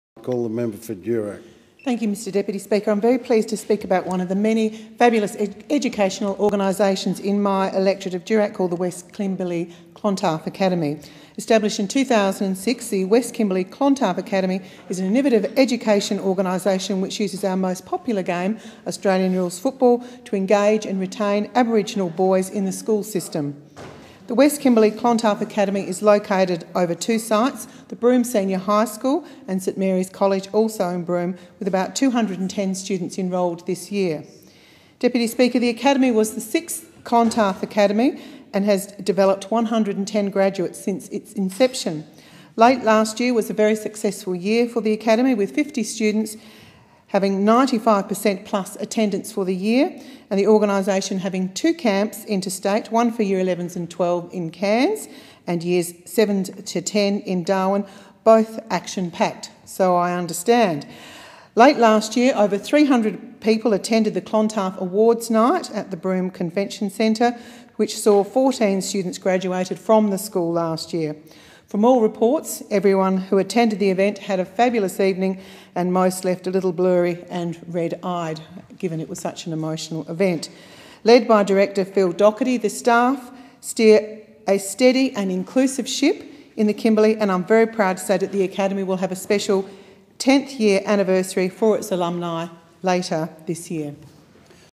West Kimberley Clontarf Academy
Established in 2006, the West Kimberley Clontarf Academy is an innovative education organisation which uses our most popular game, Australian Rules football, to engage and retain Aboriginal boys in the school system.